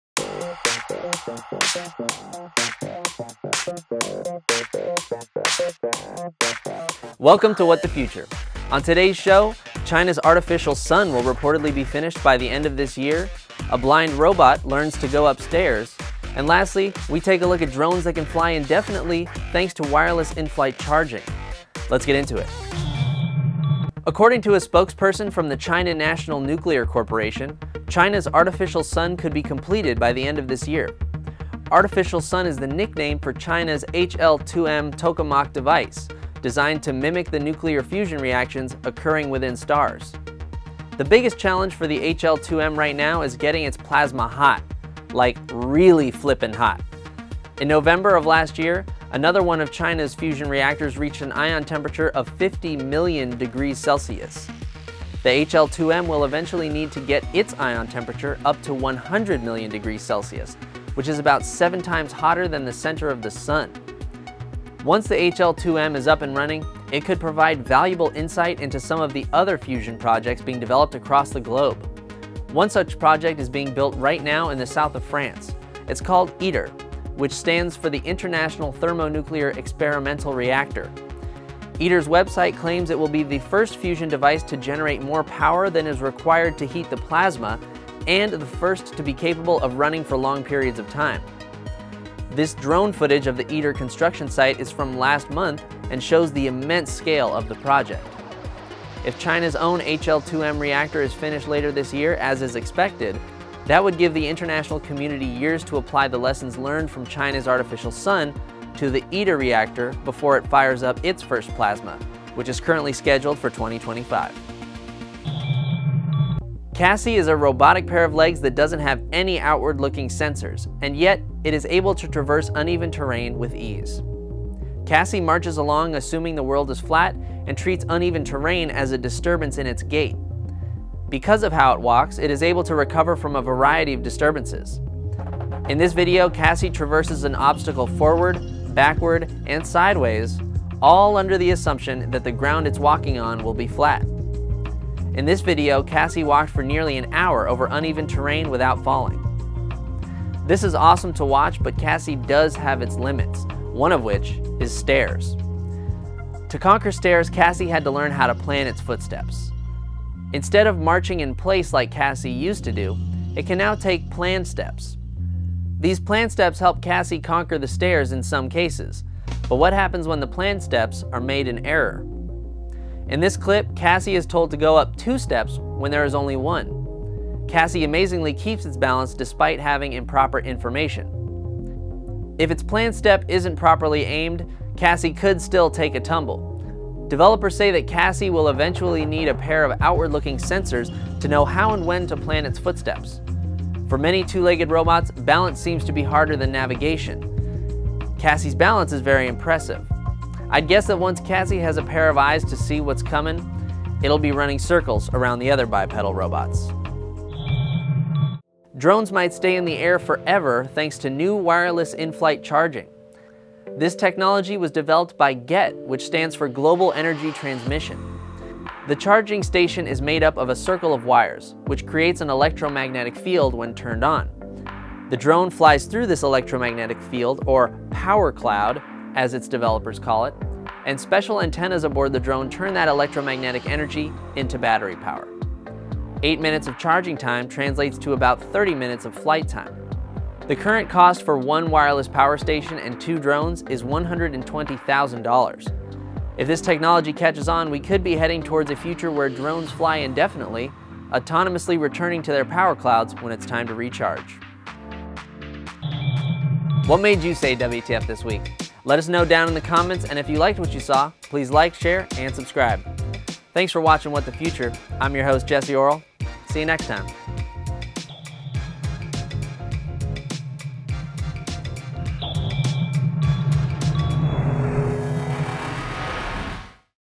China's artificial sun to be completed this year | What the Future
China's HL-2M Tokamak fusion device is expected to be finished by the end of this year.  Also, we take a look at how developers are teaching their blind robot to go up stairs, and how wireless in-flight charging could help drones fly indefinitely.